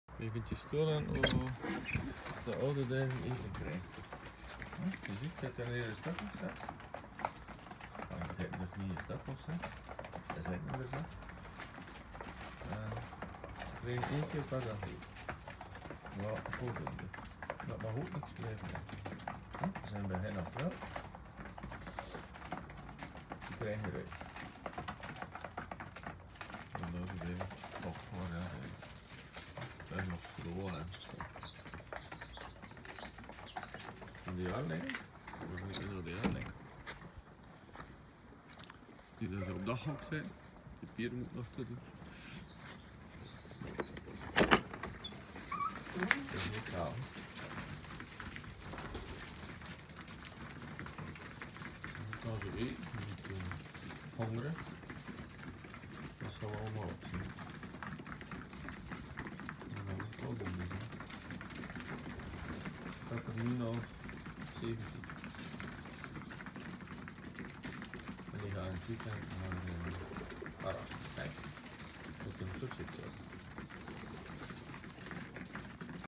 Voederen weduwnaars
Hoe voederen als ze net op weduwschap zitten? Net voor het speelseizoen.